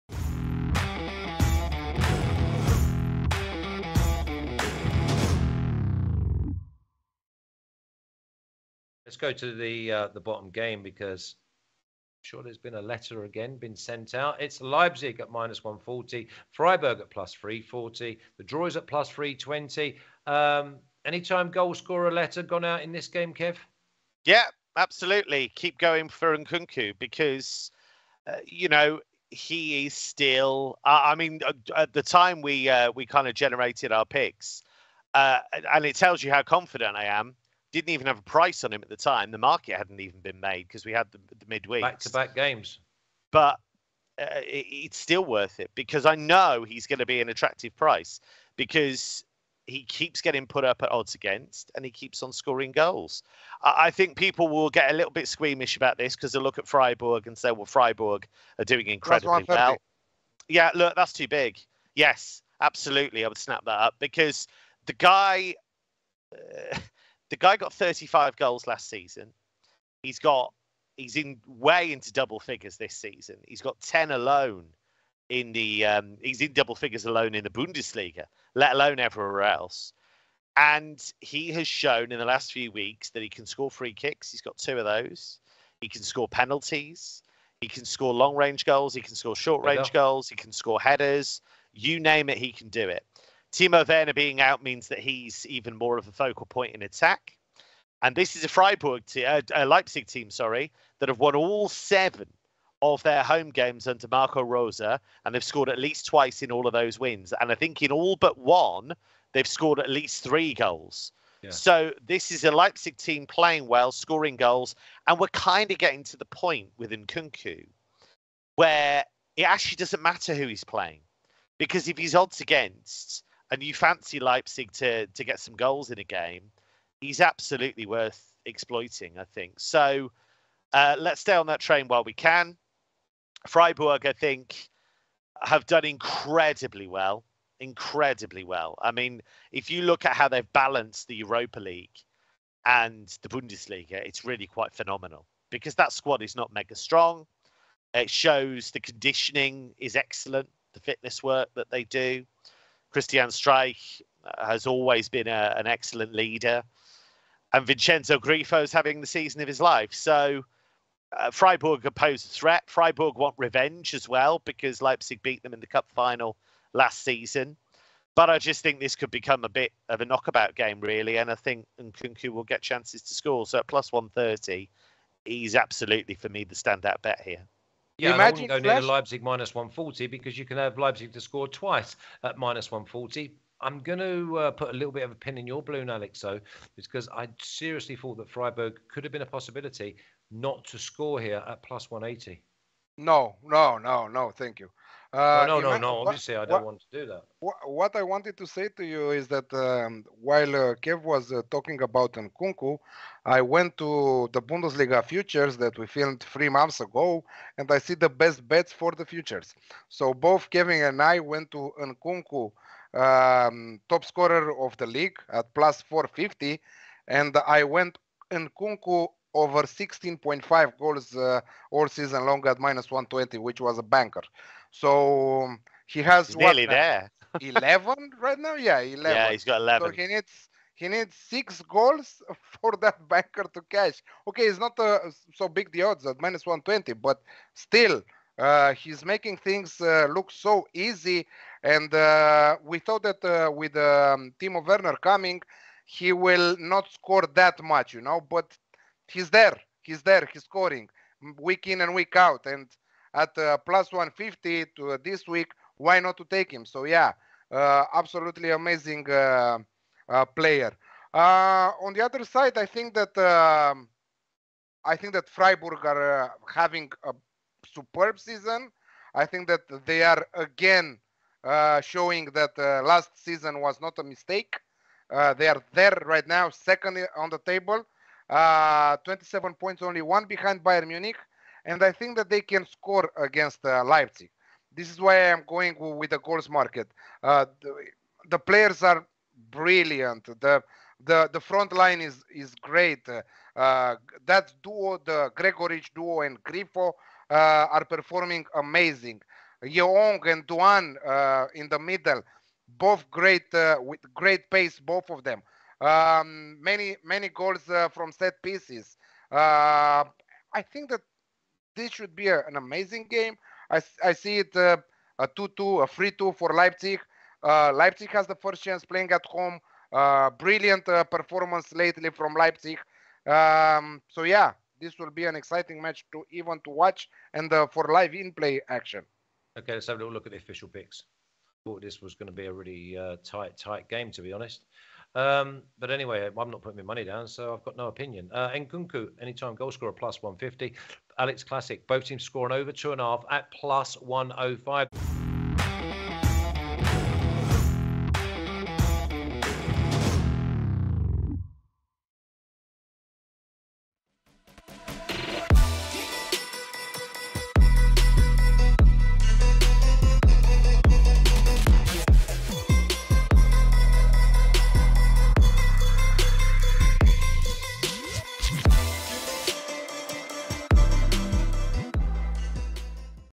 Leipzig vs Freiburg | Bundesliga Expert Predictions, Soccer Picks & Best Bets
Leipzig vs Freiburg Bundesliga game preview for November 9th, 2022.
Our betting experts Flash, Alex and Kevin preview the Bundesliga game between Leipzig vs Freiburg. They'll go over the latest sport betting trends and best value soccer odds available, and share their best soccer picks and Bundesliga expert predictions.

Connect with our betting experts on Twitter:

Flash: @Flash_37

Alex Campean: @AlexClassicTips

Kevin Hatchard: @kevinhatchard